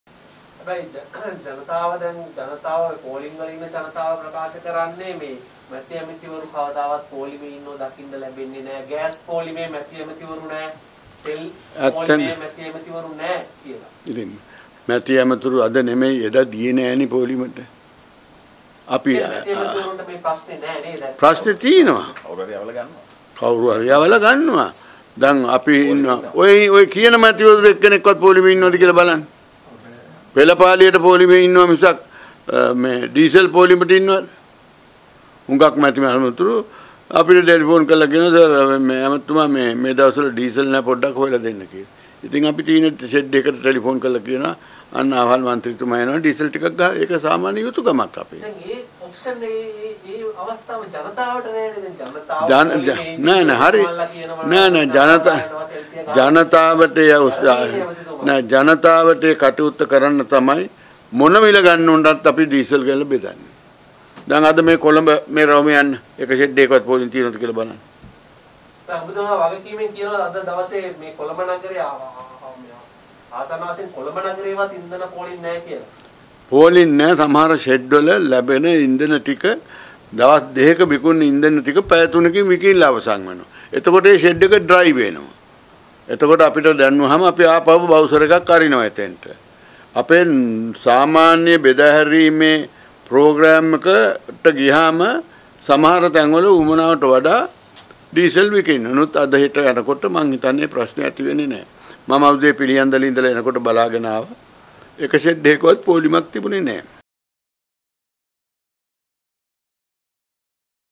මැති ඇමතිවරු කවදාවත් පෝලිමේ ඉන්නේ නෑනේ අපි කවුරු හරි යවලා ගන්නවා -ගාමිණී ලොකුගේ | Breaking.lk
මැති ඇමතිවරු කවදාවත් පෝලිමේ ඉන්නේ නෑනේ අපි කවුරු හරි යවලා ගන්නවා -ගාමිණී ලොකුගේ